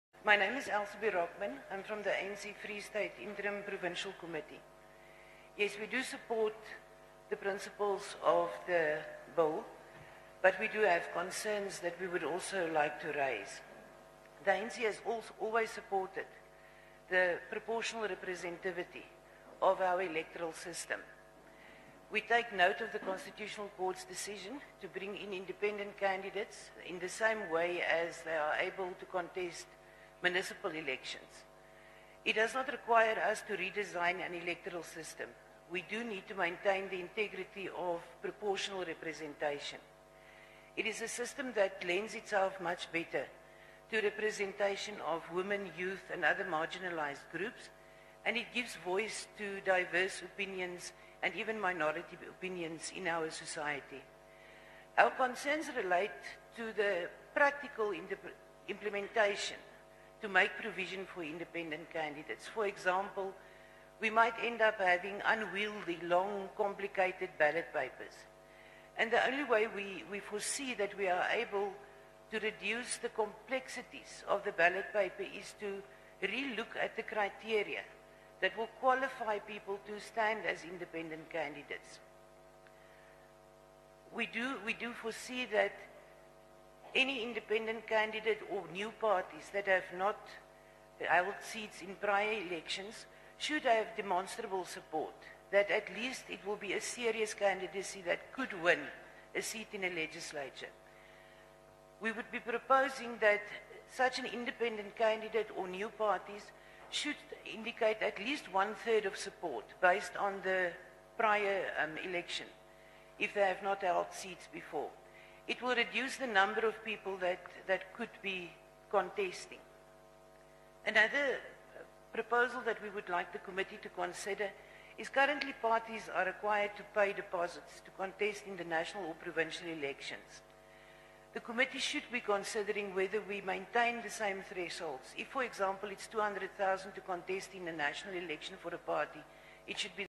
Former Free State MEC of Finance Elzabe Rockman says she supports the Electoral Amendment Bill
🟥Public Hearings on the Electoral Amendment Bill at Rockland’s Kagisanong Community Hall in Bloemfontein🟥 Former Free State MEC of Finance Elzabe Rockman says she supports the ElectoralBill but have a few concerns that need to be addressed. Says the Constitutional Court judgement doesn’t require a redesign of the electoral system. “The Bill seeks to introduce amendments to the Electoral Act 73 of 1998 after it was found to be flawed by the Constitutional Court in June 2020”